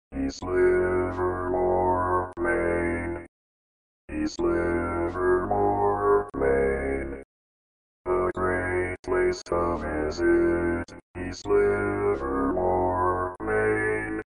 How to Say or Pronounce USA Cities — East Livermore, Maine
This video shows you how to say or pronounce East Livermore, Maine.

A computer said East Livermore, Maine. How would you say East Livermore, Maine?